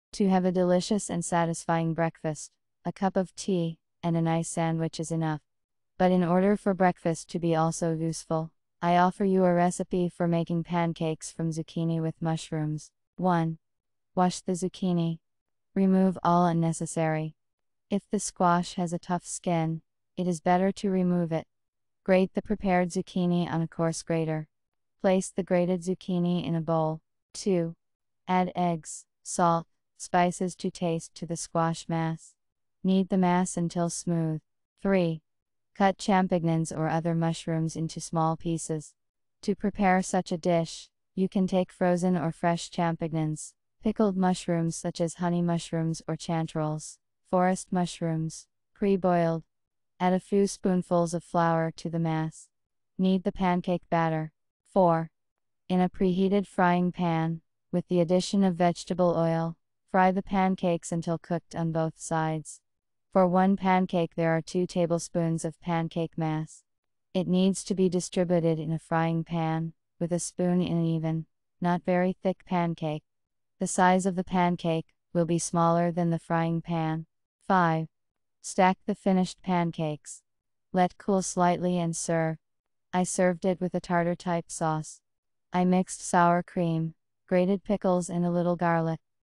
Zucchini pancakes with mushrooms
To have a delicious and satisfying breakfast, a cup of tea and a nice sandwich is enough. But in order for breakfast to be also useful, I offer you a recipe for making pancakes from zucchini with mushrooms.

Ingredients:
Zucchini — 2 Pieces,
Chicken Egg — 2 Pieces,
Mushrooms and champignons — 100 Gram,
Vegetable oil — 100 Milliliters,
Flour — 2 tablespoons (with a slide),
Salt, pepper, spices — To taste,